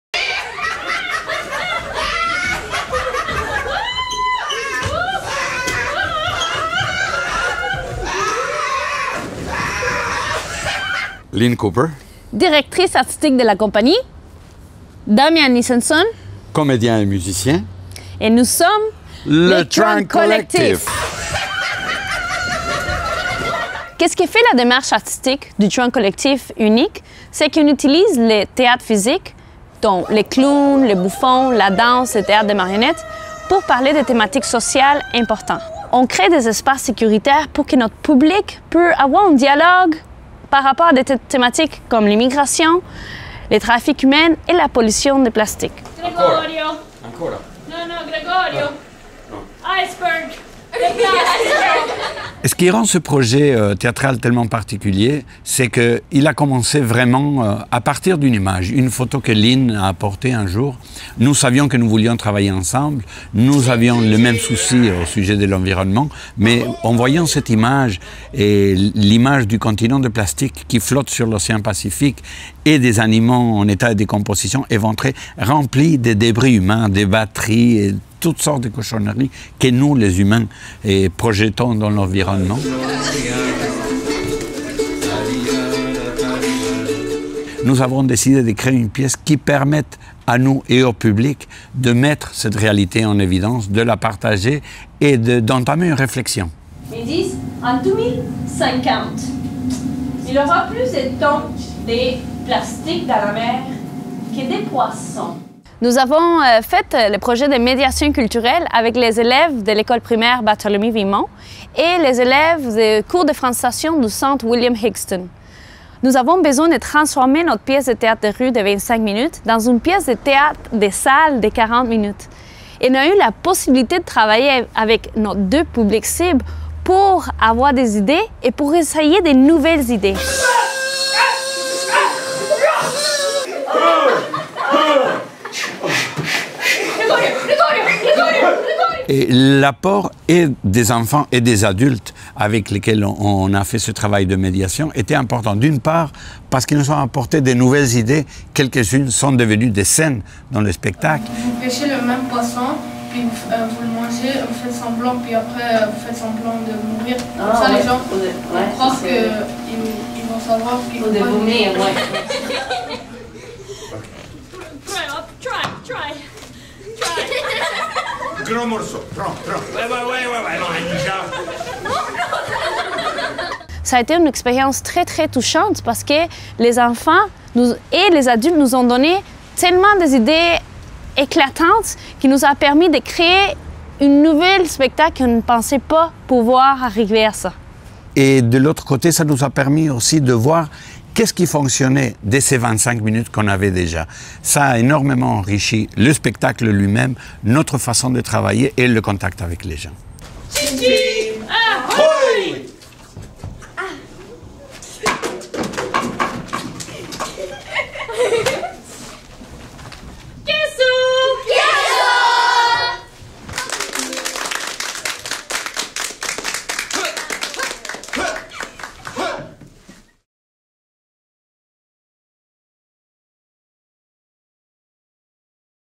La Démarche : Le Trunk Collectif
La création à travers la médiation culturelle. À l’occasion d’une résidence de création à la salle de diffusion de Parc-Extension, la compagnie de théâtre Trunk Collectif a rencontré des étudiants en francisation du centre William-Hingston et des étudiants de l’école primaire Barthélemy-Vimont dans le cadre du projet de médiation culturelle La Démarche.

Caméra et montage: Michel Pinault
Réalisation: Stéphanie Bernard